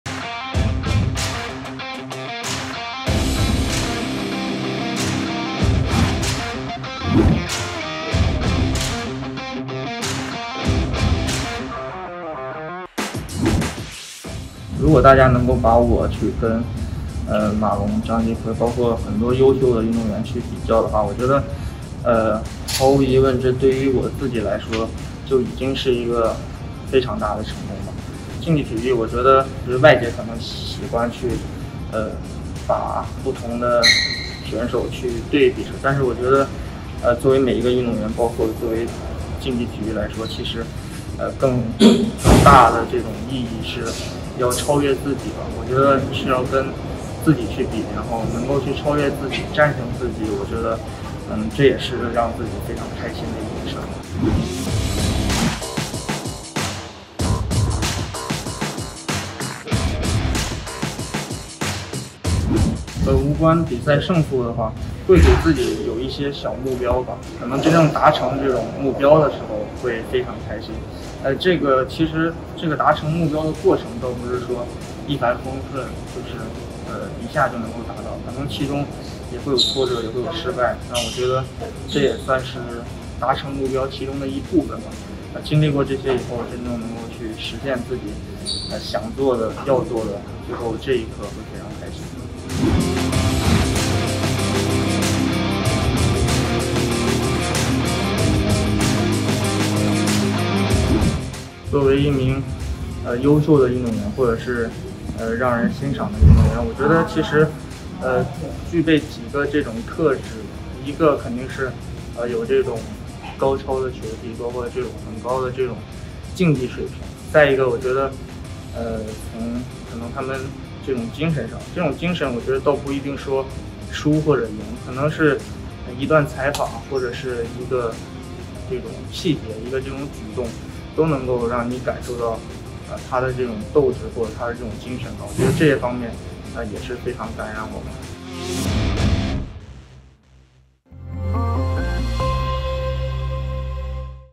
乒乓球男单世界第一 樊振东只和自己作比较
乒坛上世界第一男单、中国名将樊振东在WTT新加坡大满贯双线作战，今晚将与日本组合宇田幸矢／户上隼辅展开男双冠军争夺战，明早则再与宇田幸矢争夺男单决赛席位。点击视频，看樊振东接受记者采访分享心得。

欢迎你加入我们的Telegram频道，点击 订阅zaobao.sg新闻频道！